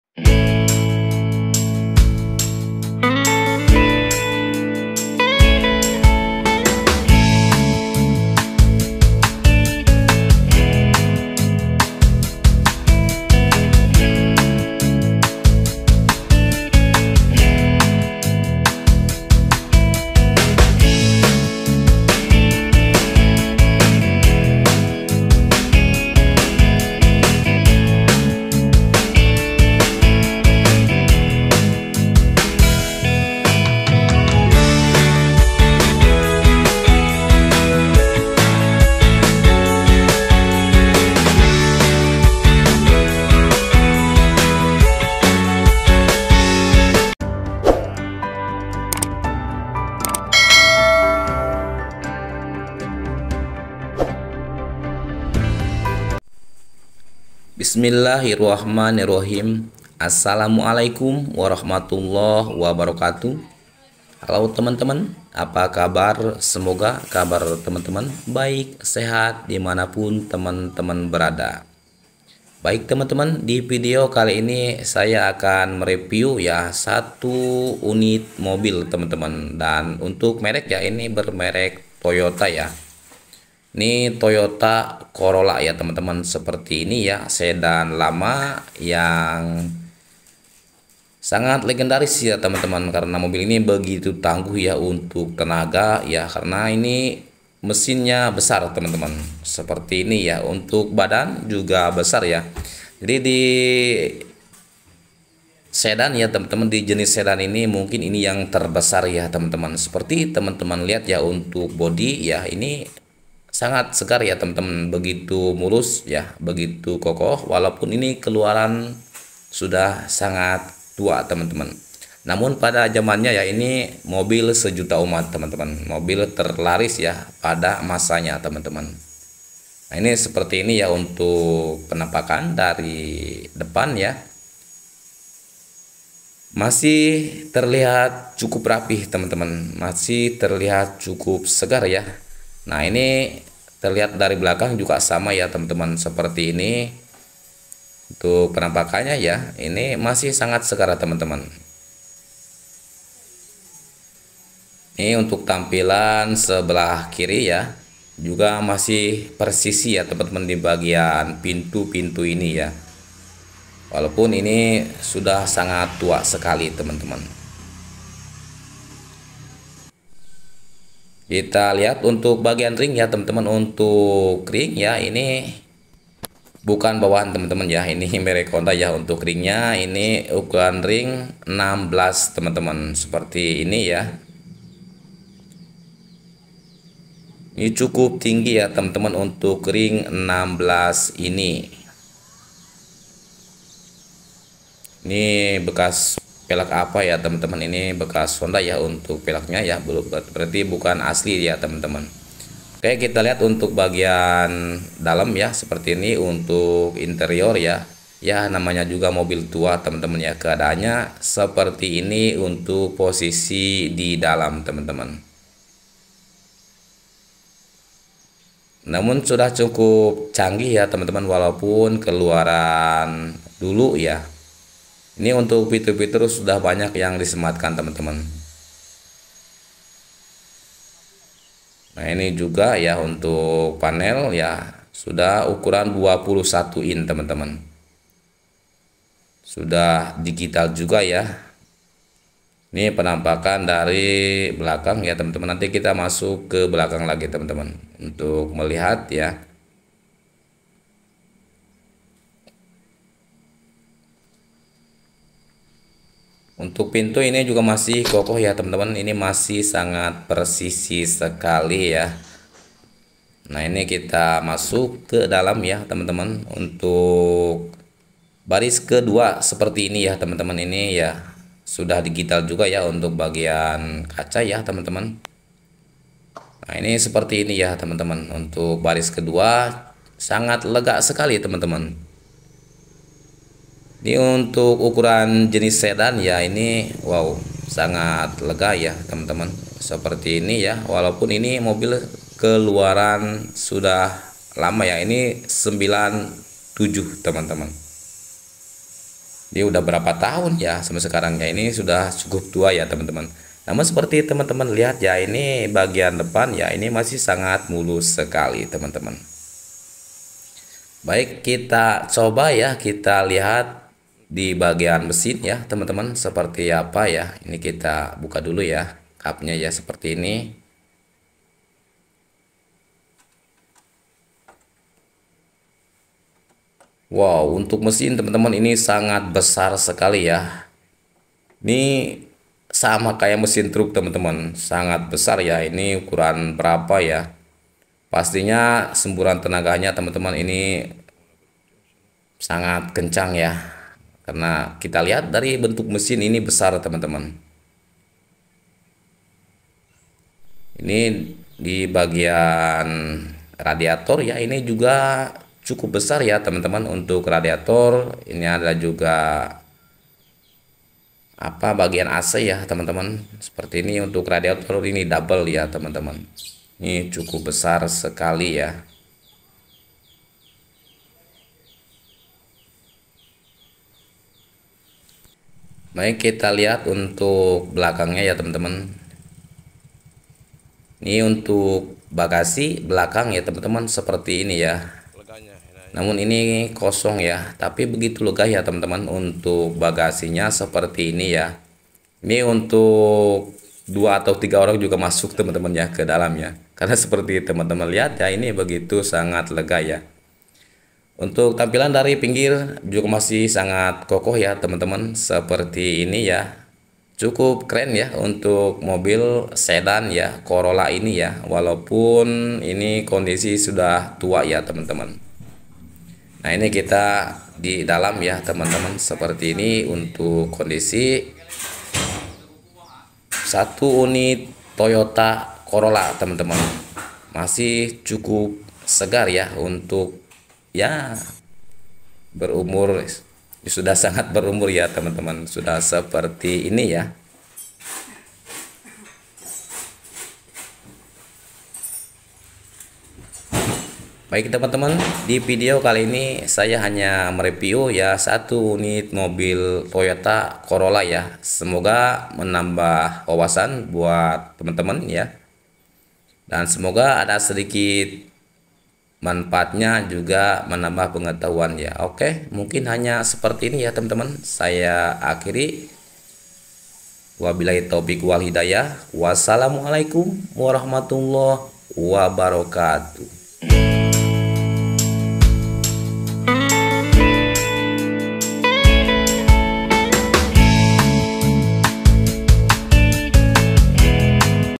Review Toyota Corolla//sang legendaris...!
Review Toyota Corolla sang legendaris mobil terlaris pada jamannya masih sangat mulus dan terlihat persisi, jenis sedan terkeren
Saksikan pidionya sampai selesai semoga menambah wawasan dan pengetahuan terimakasih